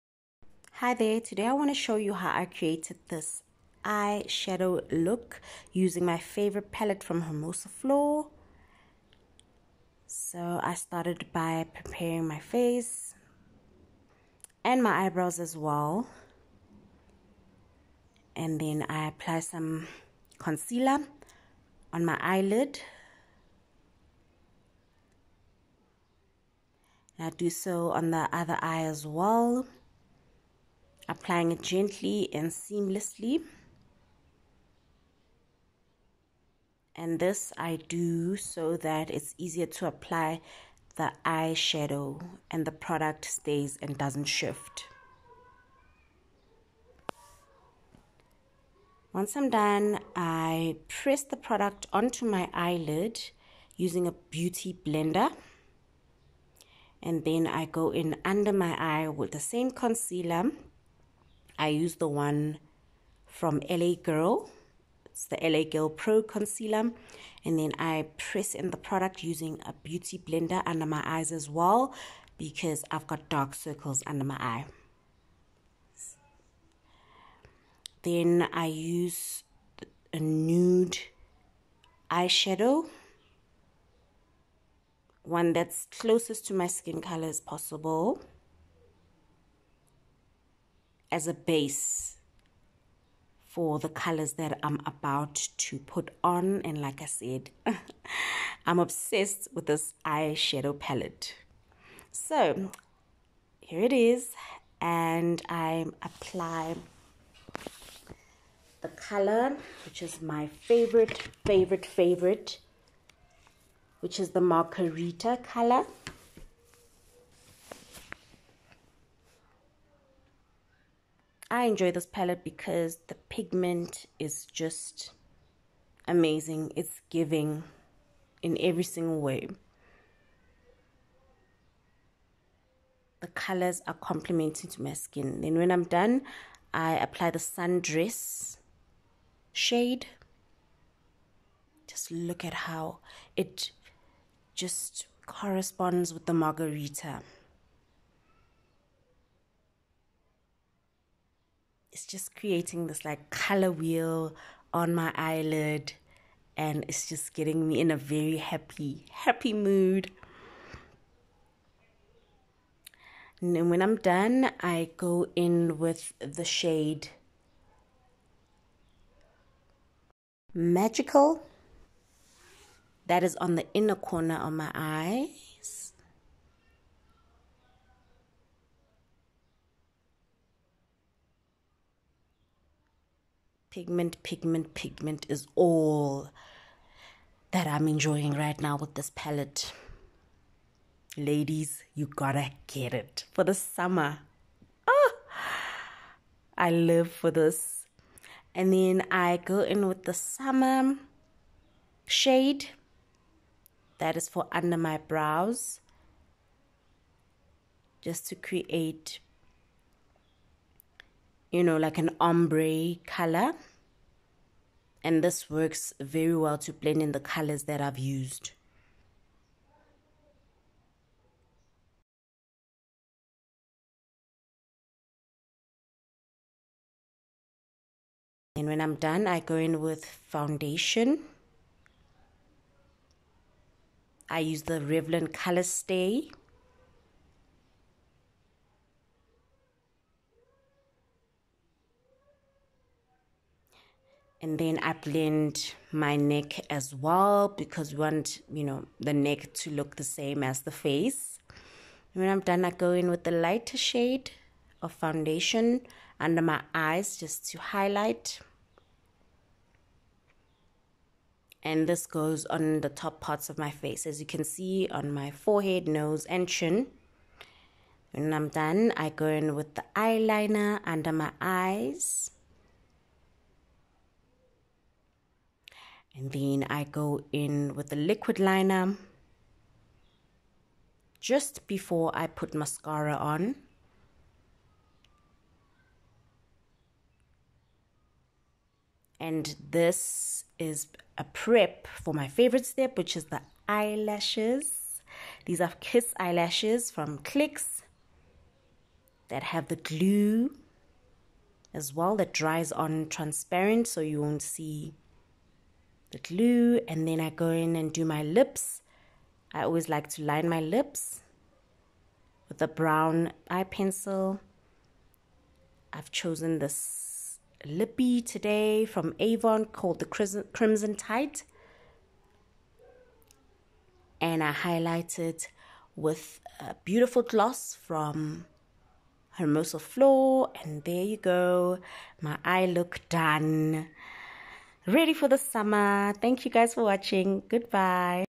How to create a bright Eyeshadow look.
I created a bright Eyeshadow look for gorgeous summer make-up look using the Hermosa flor cosmetics, colour bomb pallet and I am obsessed.

The shades I used are:
1.Magarita
2.Sundress
3.Magical
4.Summer

I am glad I tried it and ladies I recommend you should try it too. makeuptutorial makeupguide hermosaflorcosmetics beauty make-up